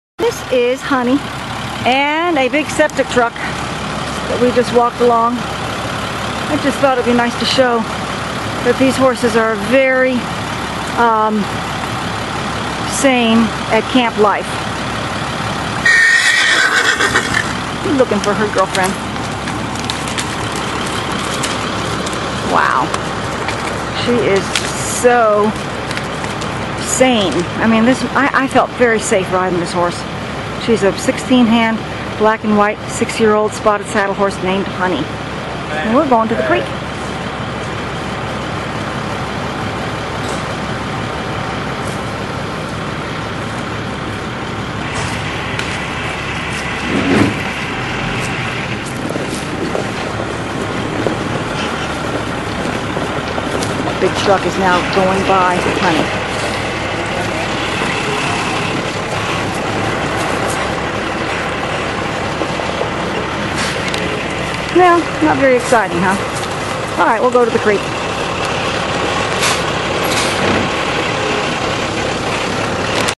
Honey black white tennessee walker mare bomb proof traffic safe septic truck
Honey came out of Savannah Tennessee.  I rode and filmed her while I was on Vacation at Bucksnort Trail ride.  I am very impressed with the demeanor and sure footedness of this sweet smooth saddling Tennessee Walking Horse Breeders and Exhibitors Association registered mare.  She is 15.2h and a horse I rode with out holding the reins so that I could photograph the other horses on the trail ride. I rode the Snowy River ride on her.  NEVER have I felt so safe on a horse.  Call Melissa (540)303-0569